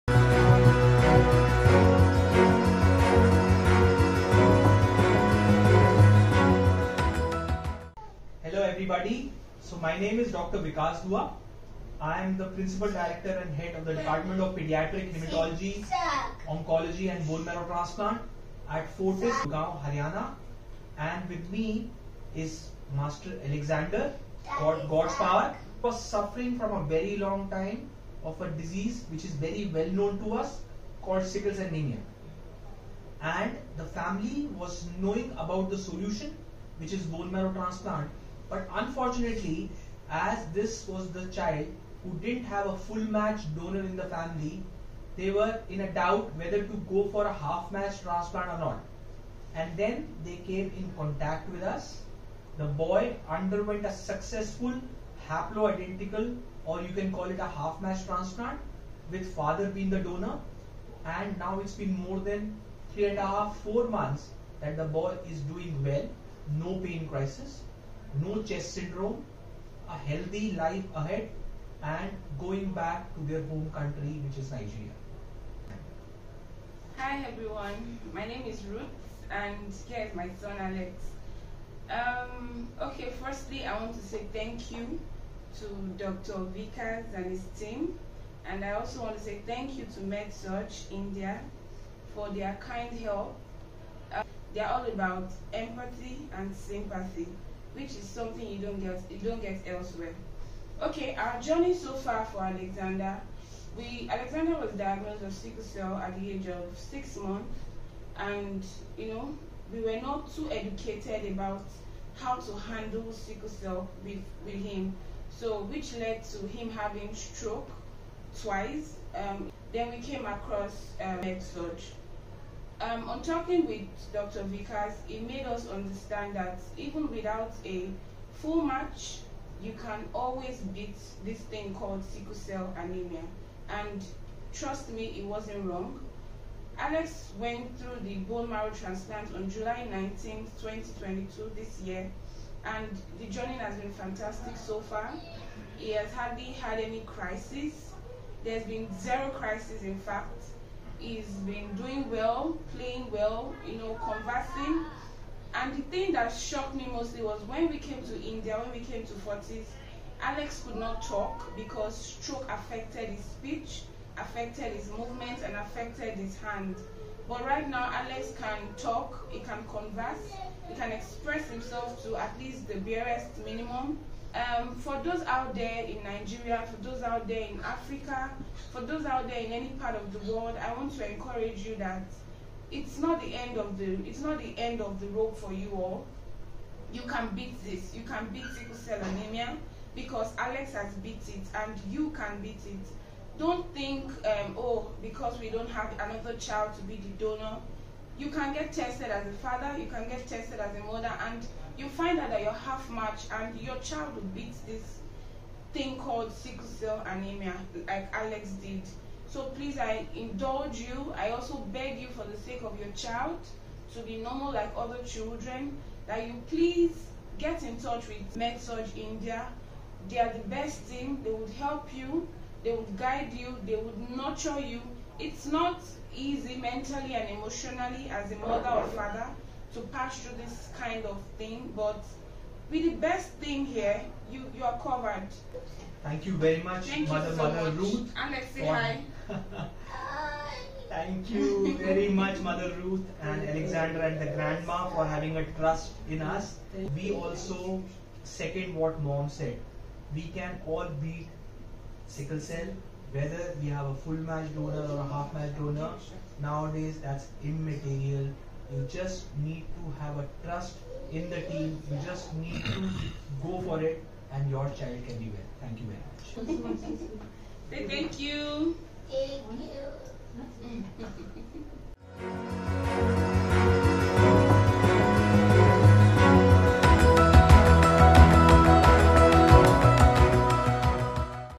Sickle Cell Anemia Treatment In India | Nigerian Patient Bone Marrow Transplant Success Story
Sickle Cell Anemia Treatment in India
Sickle cell anemia is a sickle cell disease. This is an inherited red blood cell disorder where the red blood cells in the body are not healthy enough to carry oxygen throughout the body. The normal red blood cells are round in shape and hence can easily move through the blood vessels. The abnormal once are sickle-shaped or of the shape of a crescent moon; hence the name of the disease. This particular shape makes the locomotion of the blood cells difficult within the rounded blood vessels. Hence the oxygen-carrying capacity of the blood drastically reduces.

To know more about Sickle Cell Anemia Treatment in India

Story Of Alex who Underwent Sickle Cell Anemia Treatment in India
“Before BMT Alex suffered from stroke twice which affected his speech & hand movements, now after BMT he is conversing well, moving hands with ZERO CRISIS, just like any other normal child. I want to say to my fellows that don't worry if you do not have full match donor, any parent can be half match donor and WE CAN BEAT SICKLE CELL ANEMIA, just like Alex did. ” says mother Mrs Ruth Gospower.

"Nowadays its immaterial that you have Full Match or Half Match Donor for BMT, you can still CURE SickleCell Anemia." says Dr Vikas Dua, Internationally Renowned Hematologist Oncologist and BMT Specialist.

To consult Specialized Doctor, contact our experts now!

Important Blogs Related to Sickle Cell Anemia Treatment Cost In India